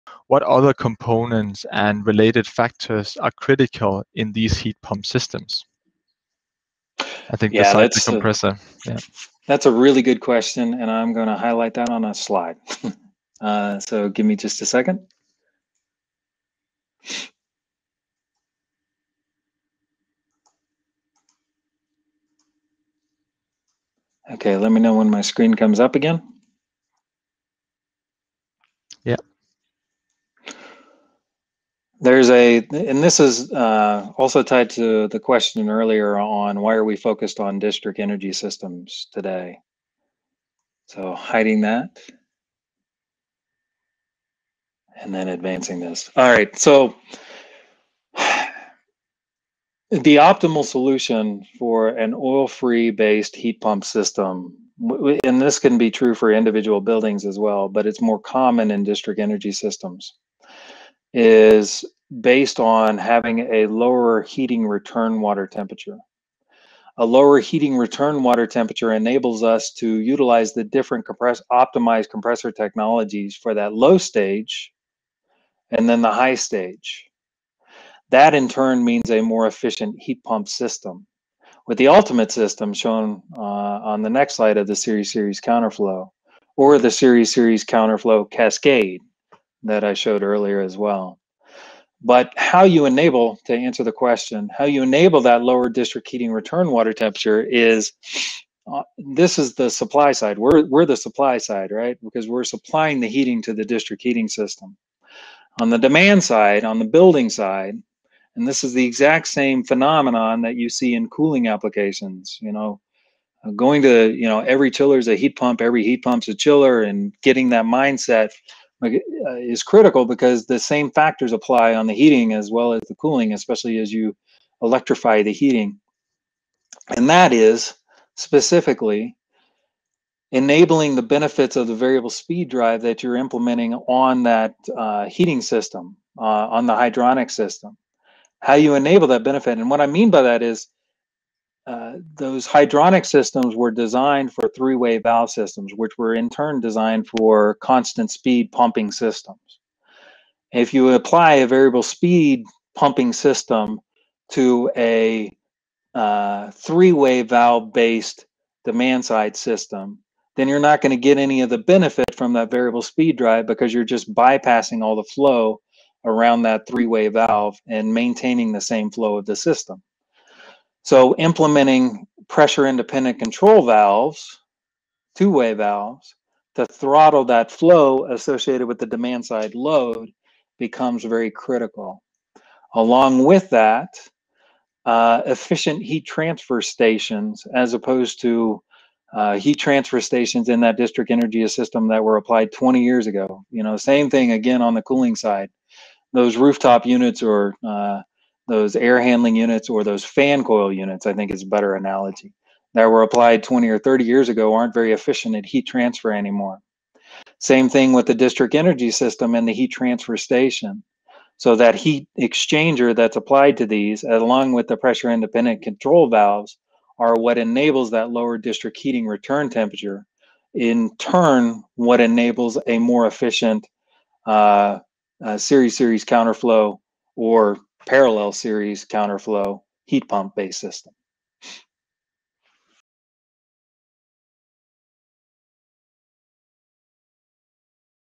What components are critical in heat pump systems? | Ask an Expert
In this clip from the Heat Pumps and Oil-Free Tech livestream, Drew Turner walks you through the critical components in the heat pump systems.

Drew talks about how the optimal solution for an oil-free heat pump system is based on having a low heating water return temperature. This temperature enables you to utilize the different optimized compressor technologies and have a more efficient heat pump system. He explains how implementing pressure independent control valves that throttle the flow and efficient heat transfer stations, along with variable speed pumping, can be critical.